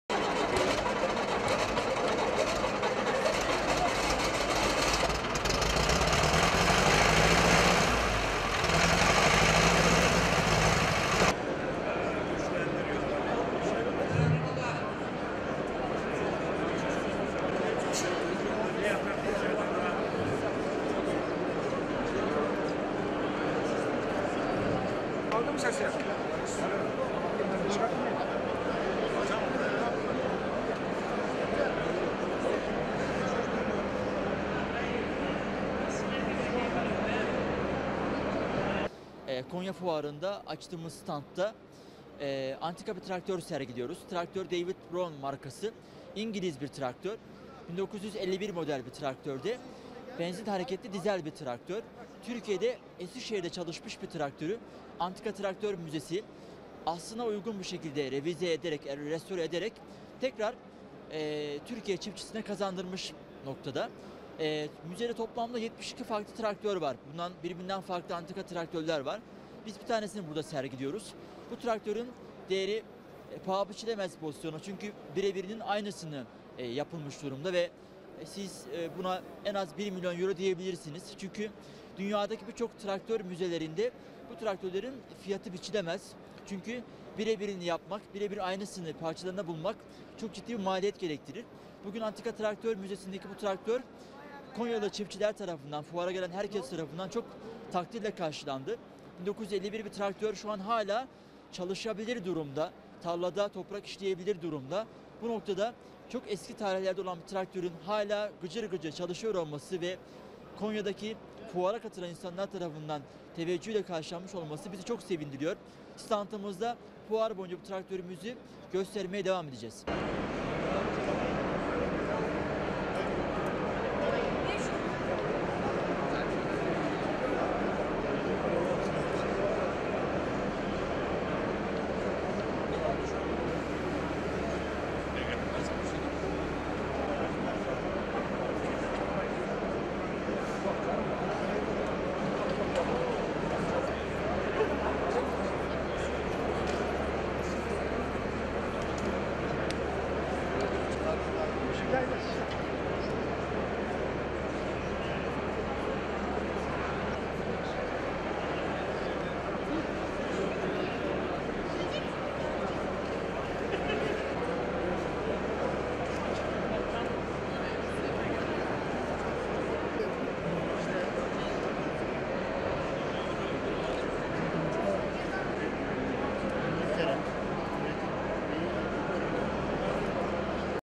DEĞERİ PAHA BİÇİLEMEZ!
Türkiye'de sadece Eskişehir'de çalıştırılan 1951 model David Brown traktör, Konya'daki Tarım Fuarı'nda sergilendi. Traktör çiftçilerden büyük ilgi gördü.